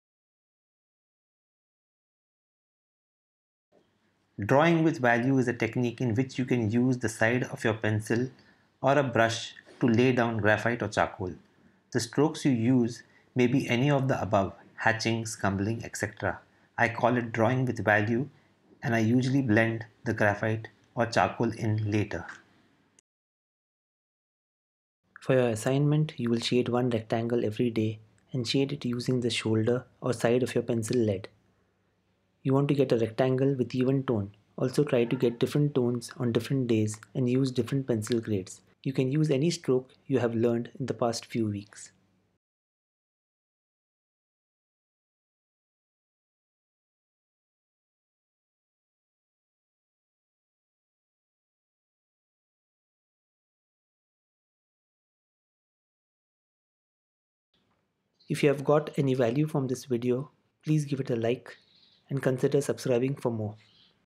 Fundamentals Of Drawing - 5 Minutes A Day - Week 16 -Artist-Technique-Mark Making-Drawing with value
In this video i talk about Drawing with value.